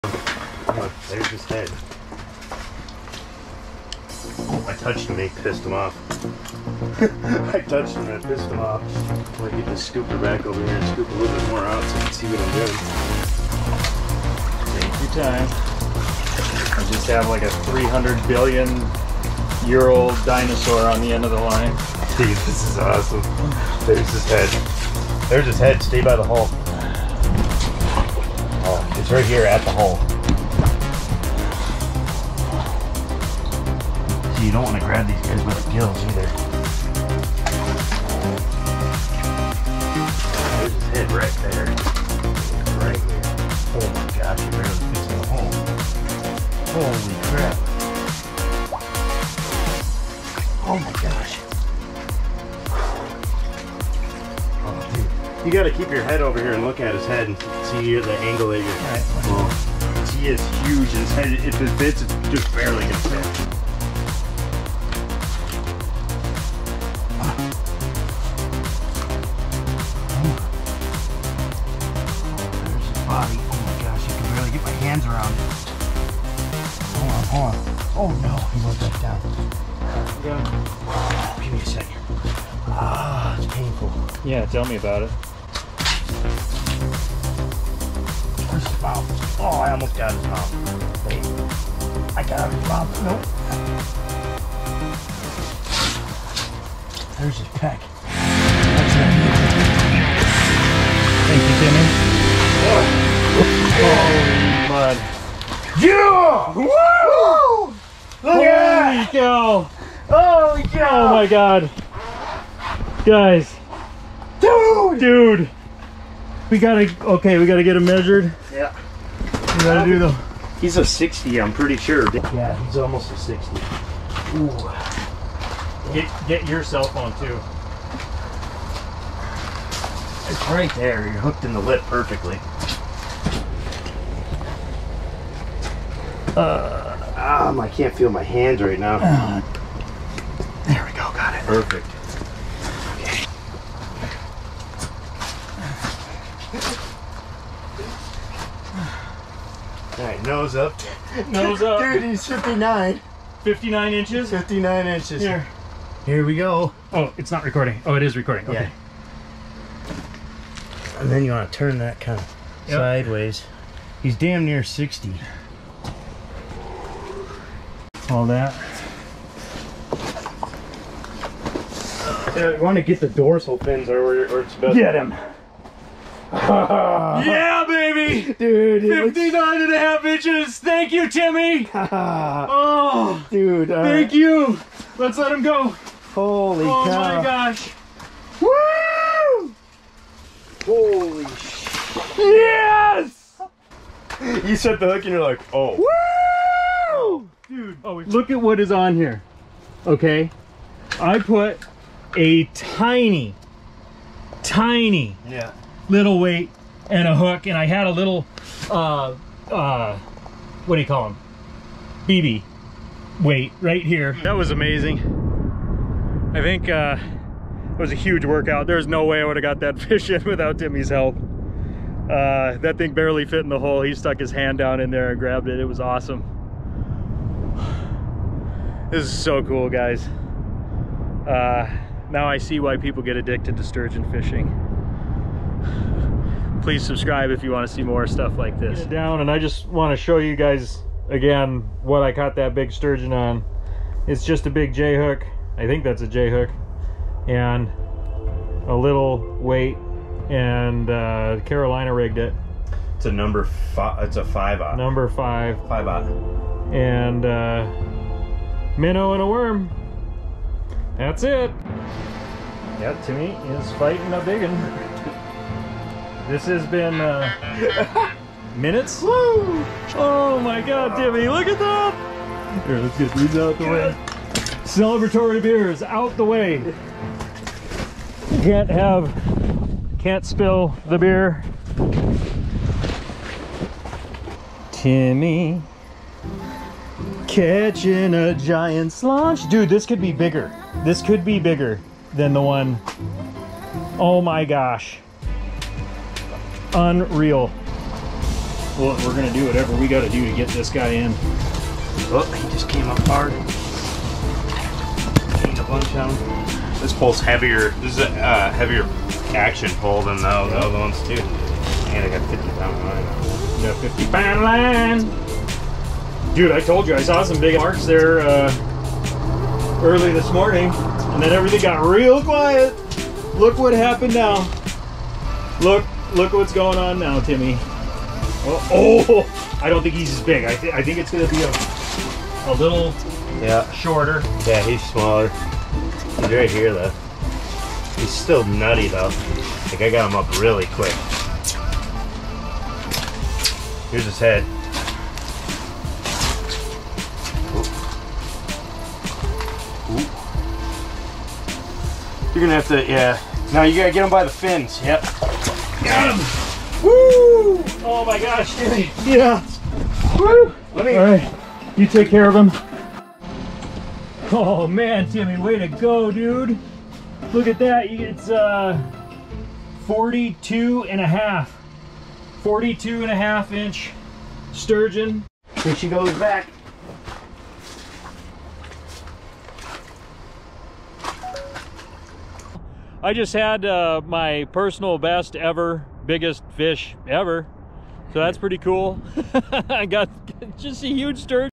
Feel the Sturge, 2 Sturgeon 1 day. Bayport MN
Ty and Timmy go Ice fishing for Lake Sturgeon on the St Croix.  How I caught a Lake Sturgeon Ice Fishing on the St Croix River.  Catching the fish of a lifetime, personal best.

Thank you to 3 Rivers Fishing Adventures book your trip

Finicky Fooler Link Amazon

Ice Rod Holder